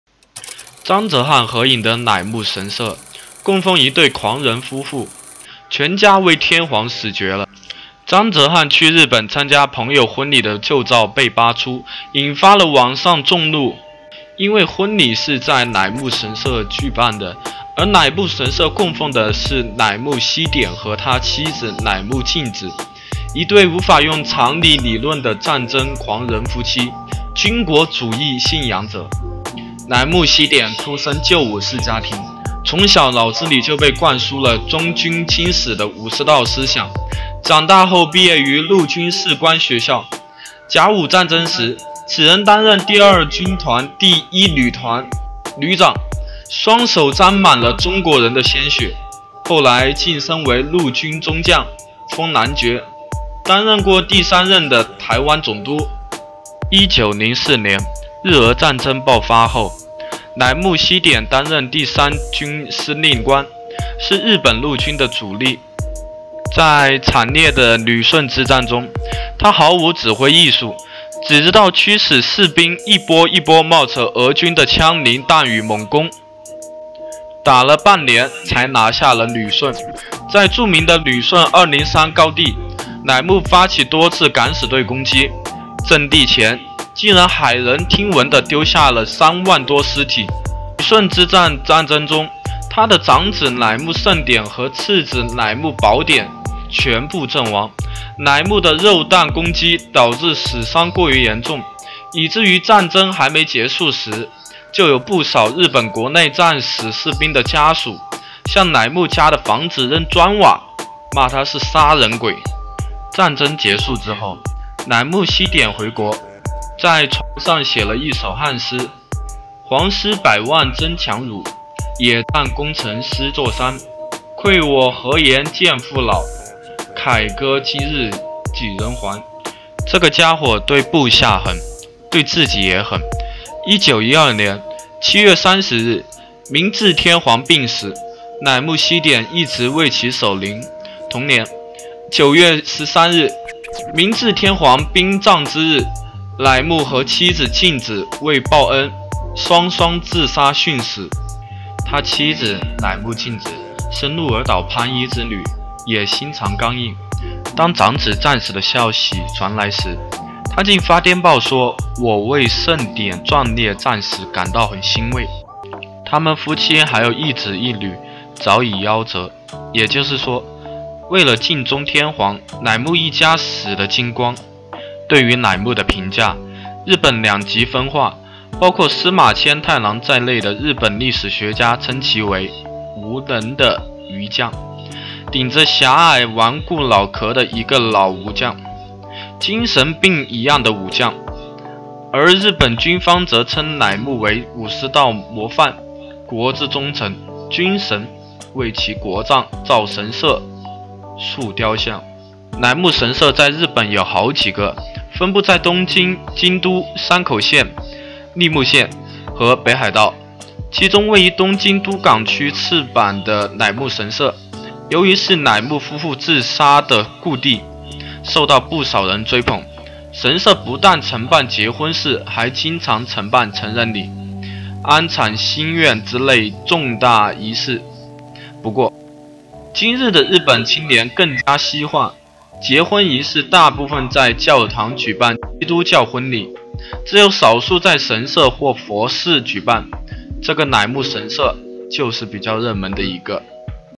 张哲瀚合影的乃木神社，到底供奉是谁？！
张哲瀚去日本参加朋友婚礼的旧照被扒出，引发了网上众怒。
为什么？因为婚礼是在乃木神社举办的，而乃木神社供奉的是乃木希典和他妻子乃木静子，一对无法用常理理喻的战争狂人夫妻。

一对狂人夫妻
乃木希典出身旧武士家庭，从小脑子里就被灌输了忠君轻死的武士道思想。长大后毕业于陆军士官学校，甲午战争时，此人担任第2军第1旅团旅长，双手沾满了中国人鲜血。后来晋升为陆军中将，封男爵，担任过第三任的台湾总督。

乃木希典
1904年日俄战争爆发后，乃木希典担任第三军司令官，是日本陆军的主力。在惨烈的旅顺之战中，他毫无指挥艺术，只知道驱使士兵一波一波冒着俄军的枪林弹雨猛攻，打了半年才拿下了旅顺。在著名的旅顺203高地，乃木发起多次敢死队攻击，阵地前竟然骇人听闻地丢下了三万多尸体。